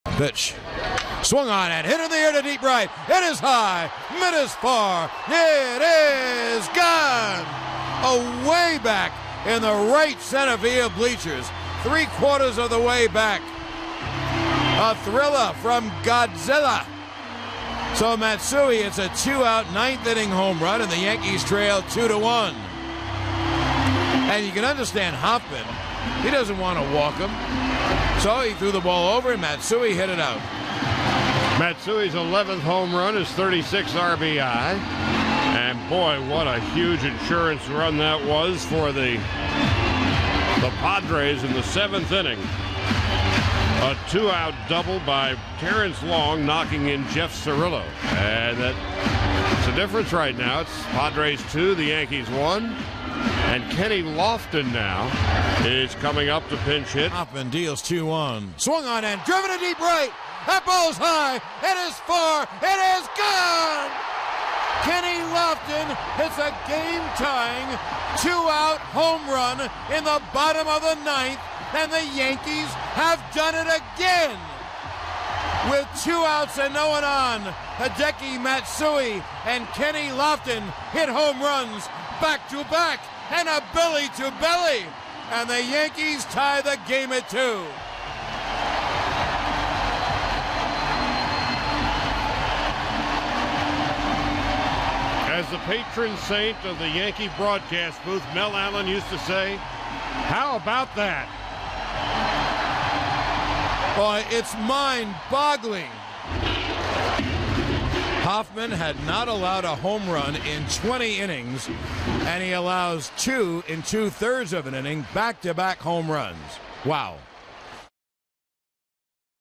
Matsui, Lofton hit back-to-back HRs to tie the game in the 9th
6/13/04: Down 2-0 with two outs in the 9th against Trevor Hoffman, Hideki Matsui and Kenny Lofton hit back-to-back homers to tie the game